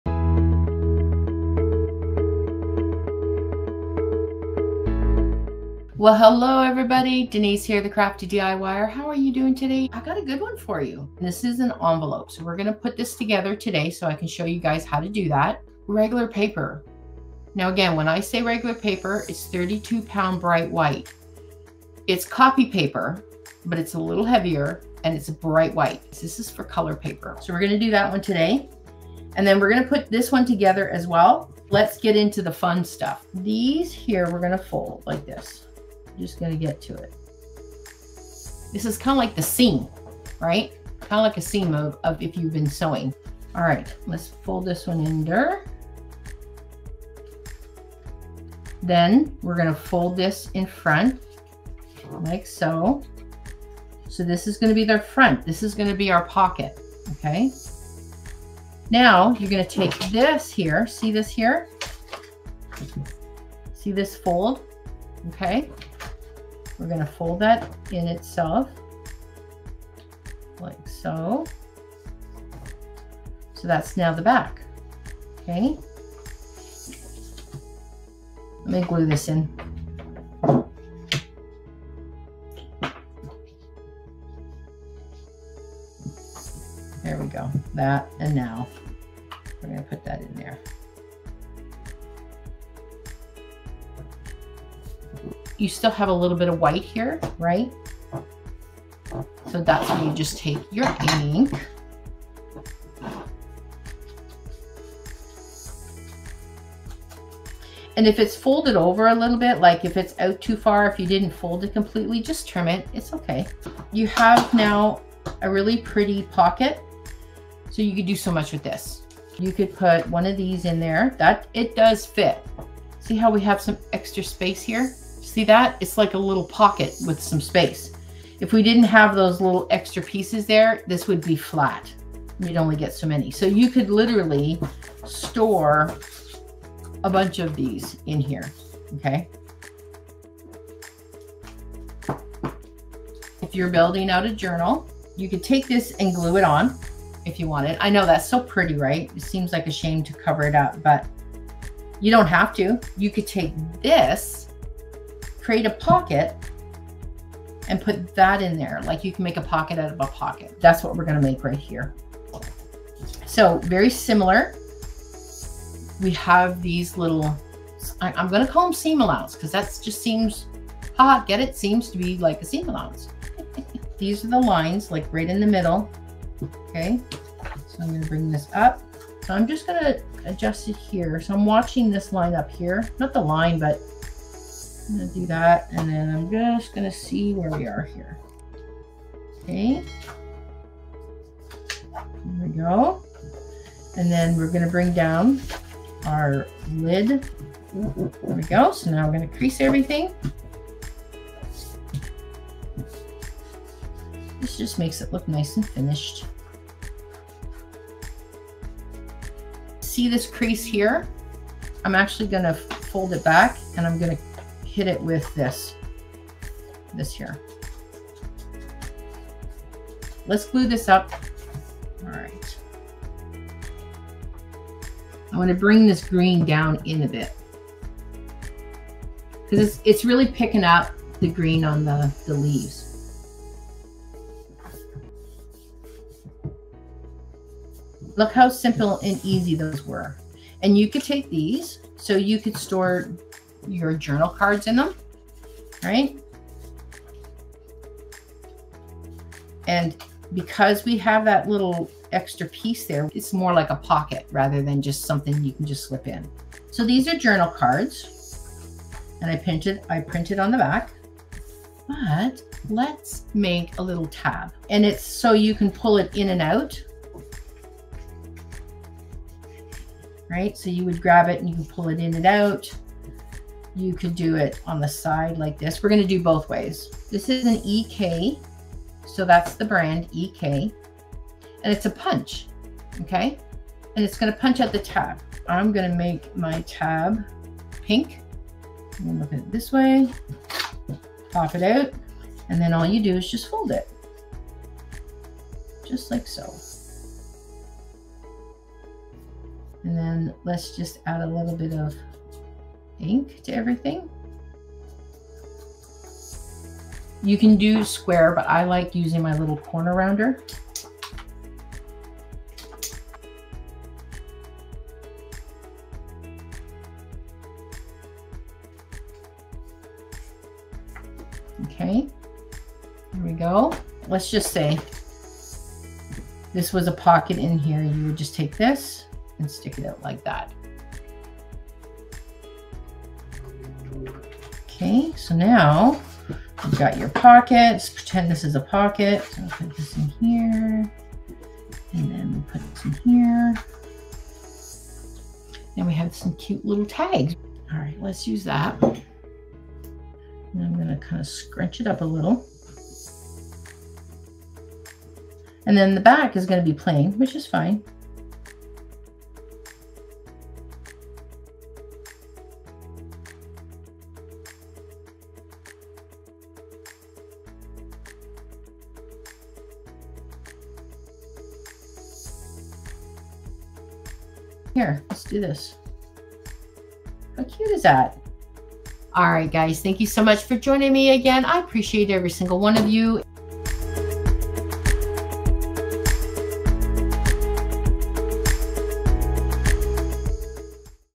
Learn easy junk journal pockets and tags In this step by step tutorial
Learn easy junk journal pockets, tags and an envelope, In this step by step tutorial, Paper crafting is fun and easy on the budget you can find the Victorian Journal Printable Bundle I used today

Get my free folio kit printable: including a step by step video showing you how to put it together!

 INQUIRIES!